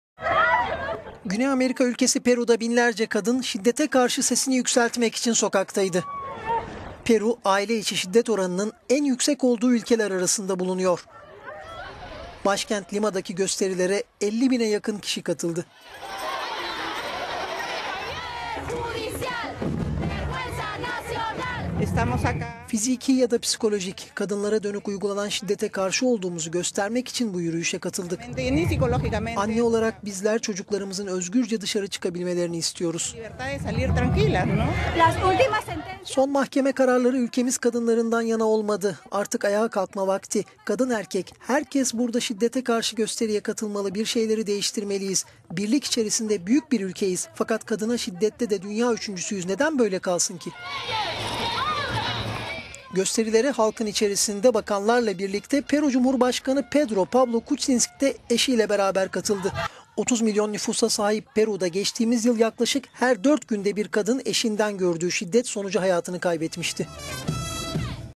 Perulu kadınlar şiddete karşı "yeter" dedi
Aile içi şiddet oranının en yüksek olduğu ülkeler arasında bulunan Peru'da binlerce kadın şiddete karşı sesini yükseltmek için sokaktaydı. Güney Amerika ülkesinin başkenti Lima'daki gösterilere 50 bine yakın kişi katıldı.

"Fiziki ya da psikolojik kadınlara dönük uygulanan şiddete karşı olduğumuzu göstermek için bu yürüyüşe katıldık. Anne olarak bizler çocuklarımızın özgürce dışarı çıkabilmelerini istiyoruz." (Carmela Garcia, ev hanımı)

"Son mahkeme kararları ülkemiz kadınlarından yana olmadı. …